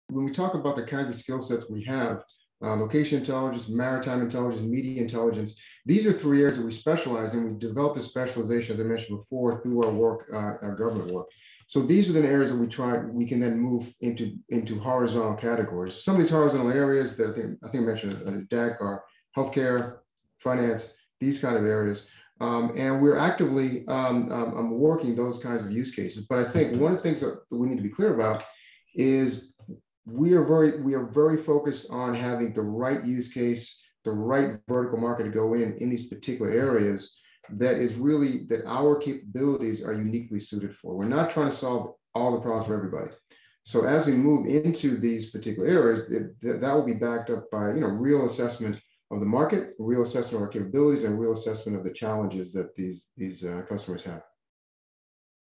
Fireside Chat: BigBear.ai Replay 2
Fireside Chat with CEO, CFO, and CTO of BigBear.ai and the Founding Managing Partner of GigCapital4, Inc. - November 17, 2021